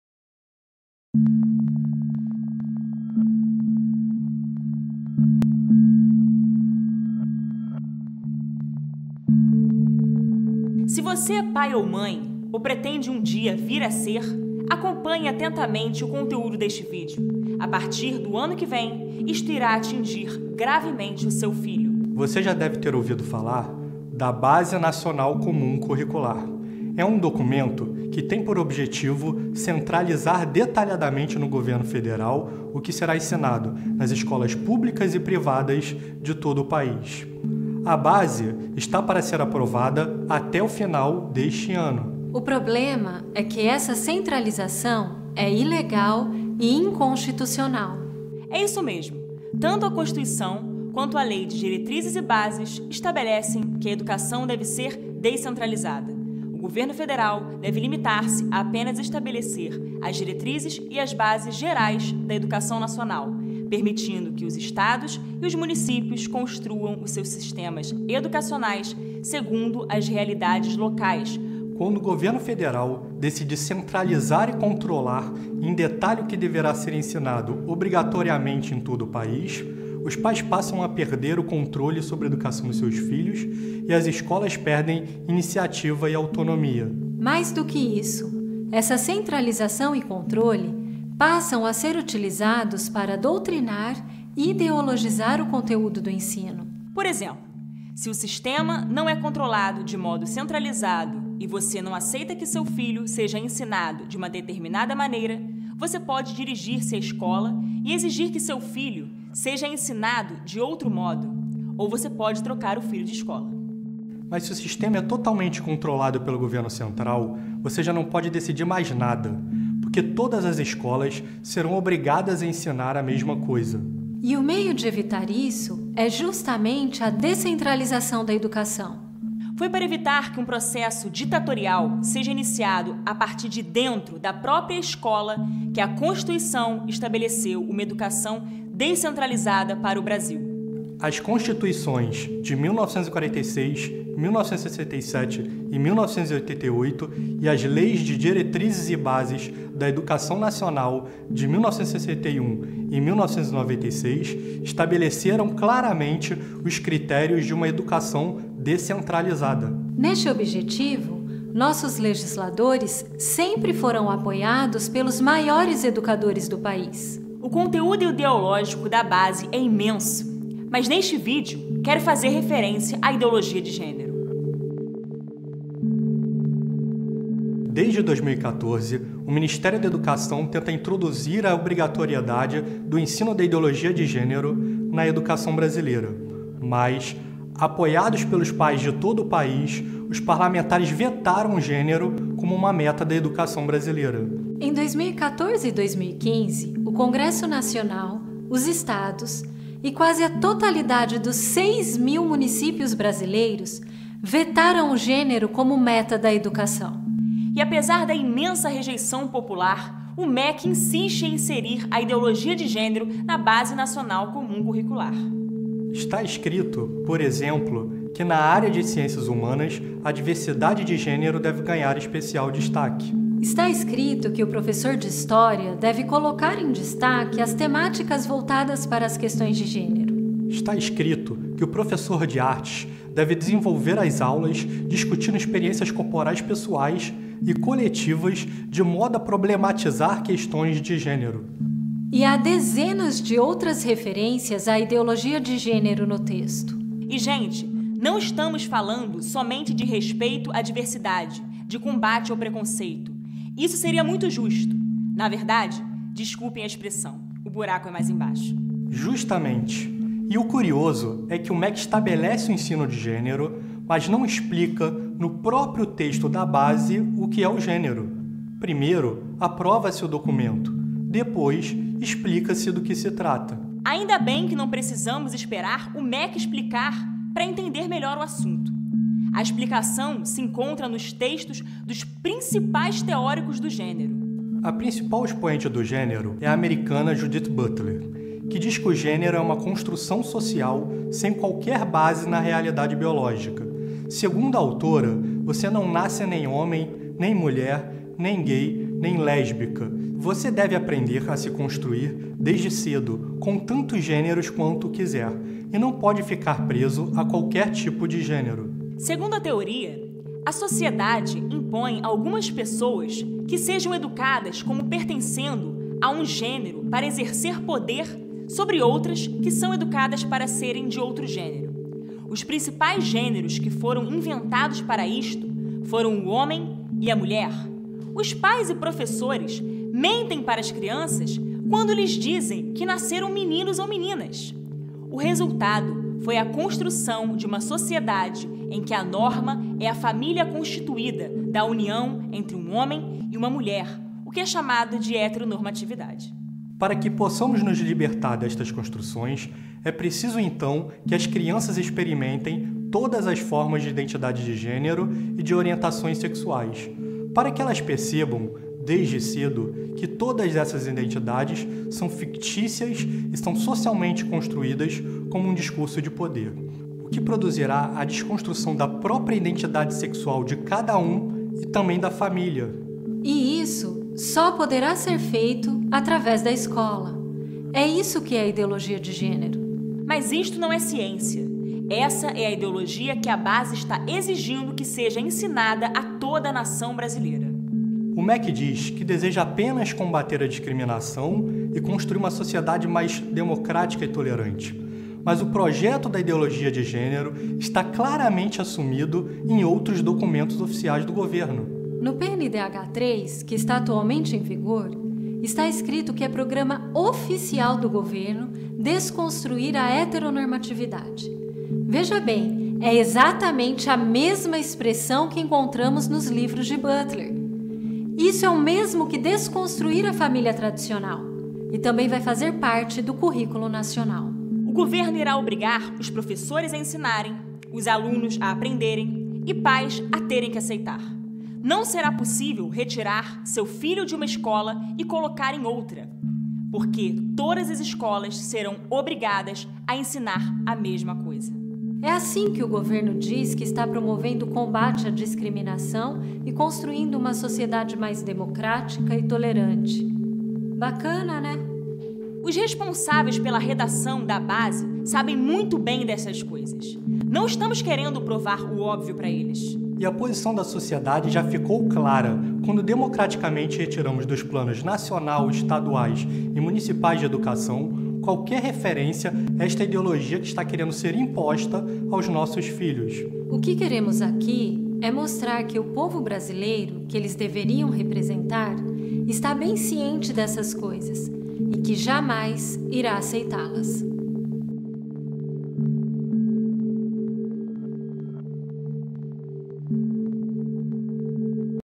A base nacional comum curricular é uma FRAUDE e fere direitos legitimados na LDB 1996
O Presidente INSANO e ilegítimo Michel temer quer destruir tudo que os maiores pensadores da Educação preconizava para DESCENTRALIZAR. Como Anísio Teixeira .. Darcy Ribeiro entre outros.

Diga não a esta aberração!